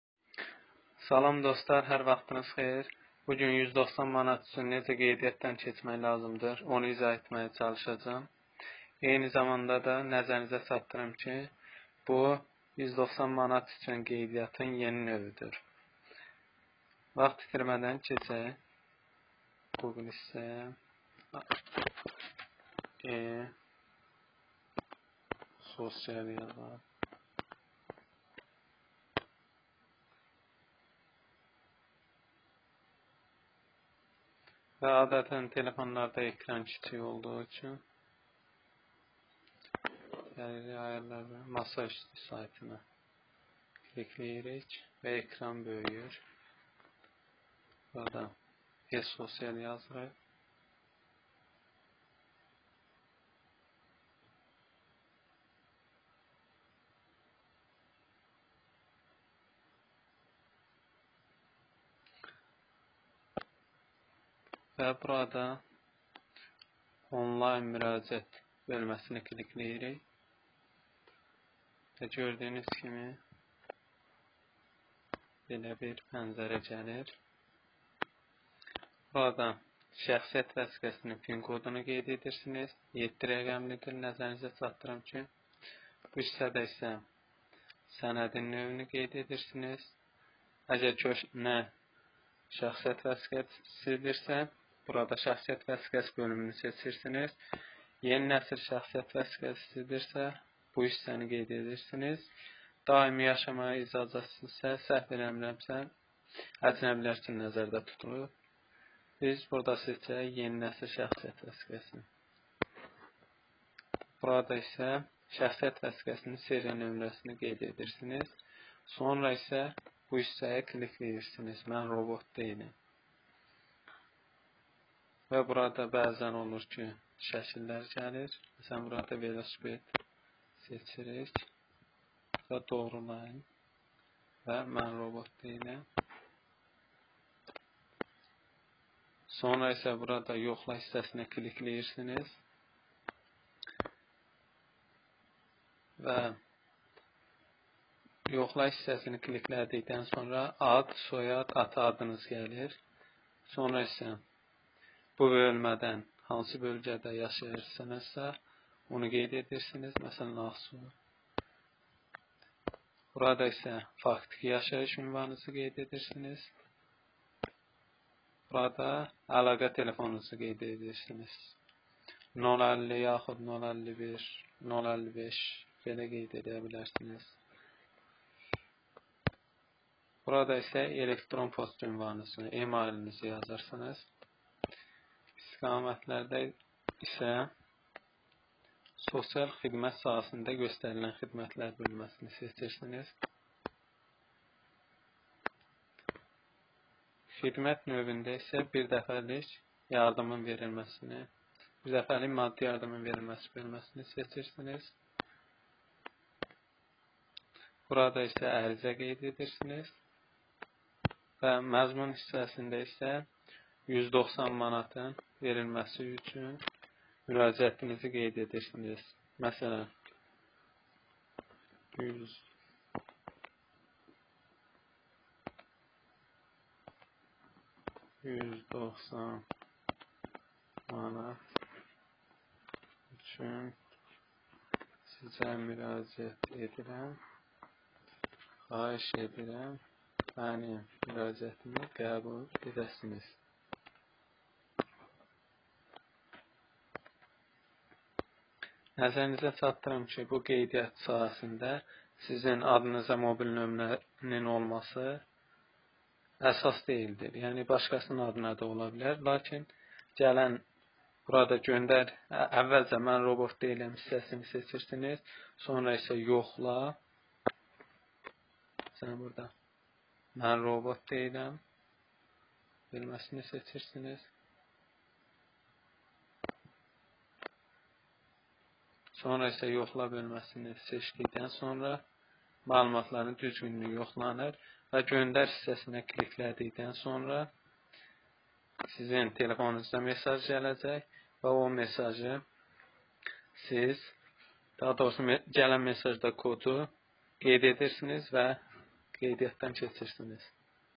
190 manat üçün qeydiyyatdan keçməyin yeni qaydasını izah etmişəm.
190 manat üçün qeydiyyatdan necə keçmək lazımdır onu izah etmişəm ,çətinlik çəksəniz qeydiyyat zamanı yorumlarda yazarsanız   kömək etməyə çalışacam.

Video xoşunuza gəldisə bəyənməyi və abunə olmayı unutmayın.